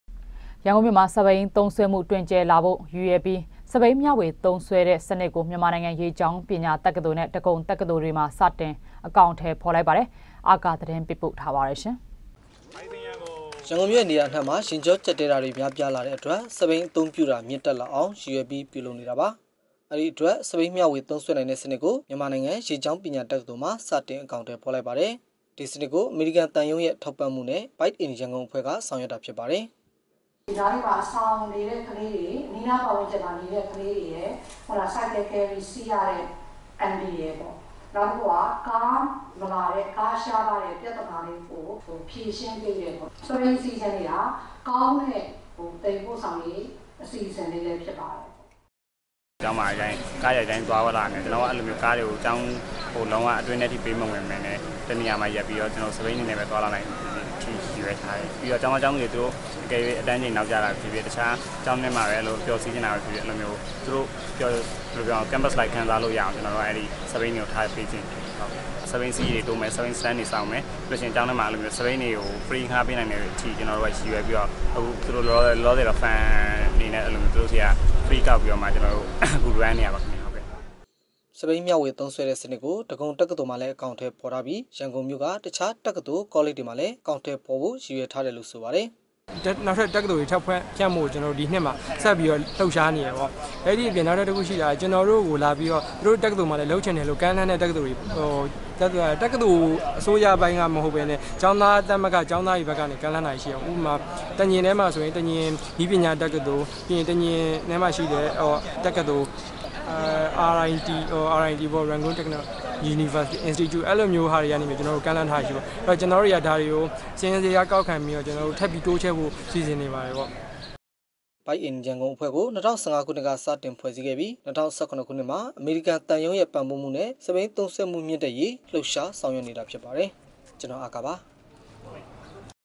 DVB - စက္ဘီးမ်ွေဝသံုးစြဲတဲ့ စနစ္ တကၠသိုလ္ေတြမွာ စတင္အေကာင္အထည္ေဖာ္
ရန္ကုန္ျမိဳ ့မွာ စက္ဘီးသံုးစြဲမွဳ တြင္က်ယ္လာဖို ့ရည္ရြယ္ျပီး စက္ဘီးမ်ွေဝသံုးစြဲတဲ့ စနစ္ကို ျမန္မာနိုင္ငံေရေၾကာင္း ပညာ တကၠသိုလ္နဲ ့ ဒဂံုတကၠသိုလ္တို ့မွာ စတင္ အေကာင္အထည္ေဖာ္လိုက္ပါတယ္။ ကိုအာကာ သတင္းေပးပို ့ထားပါတယ္။